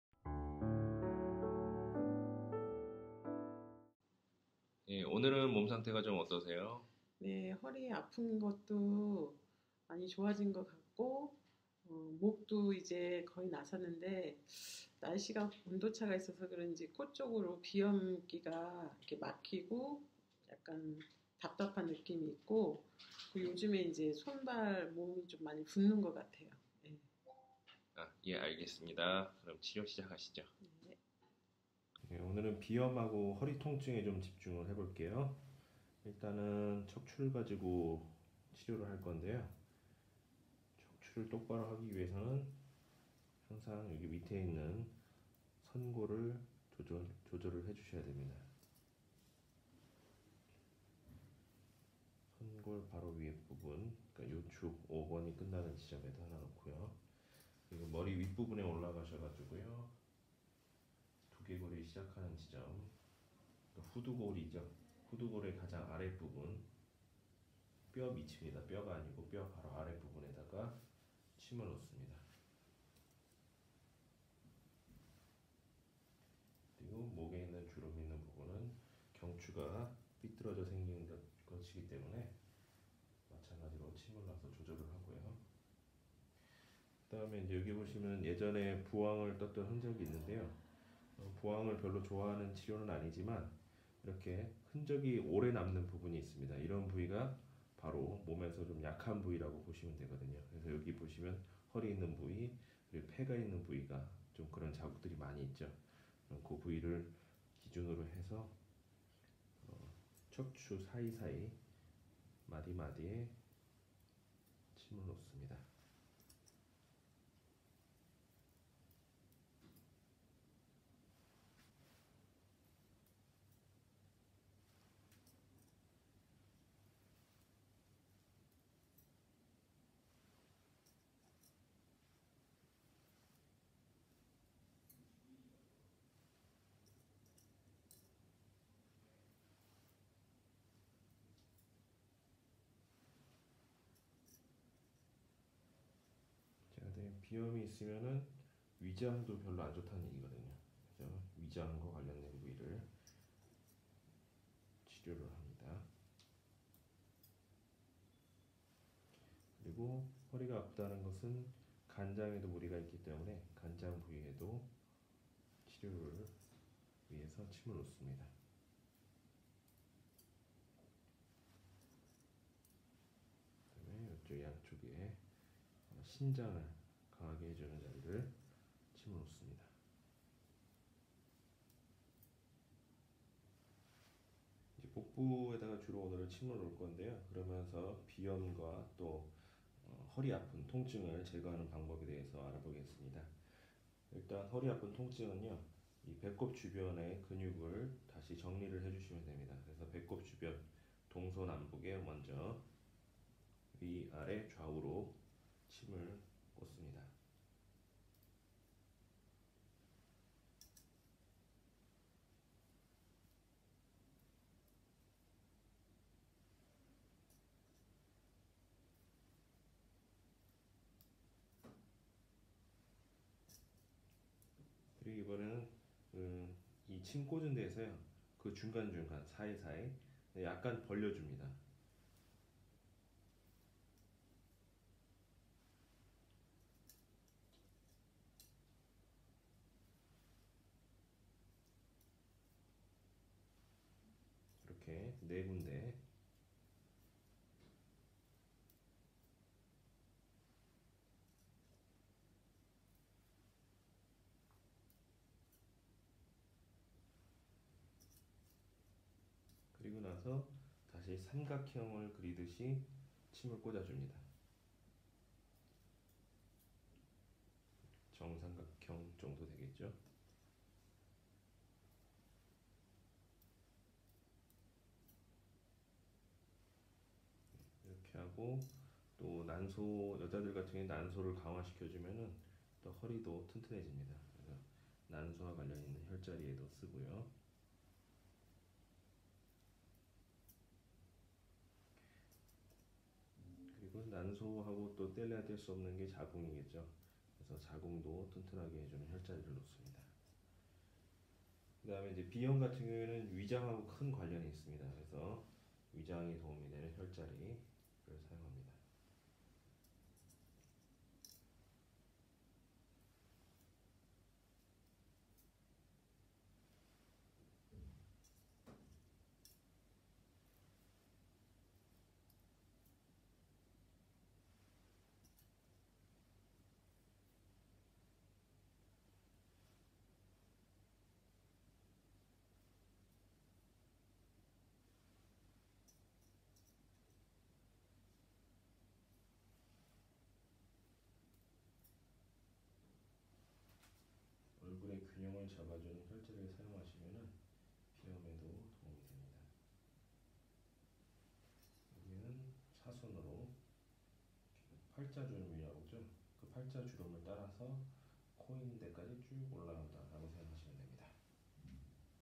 임상기록 - 눈물, 허리통증, 비염 9편 - 힐링침 / 균형요법 / 내손은약손 - Acupuncture Points
더 자세한 정보는 제 블로그에서 얻을 수 있습니다.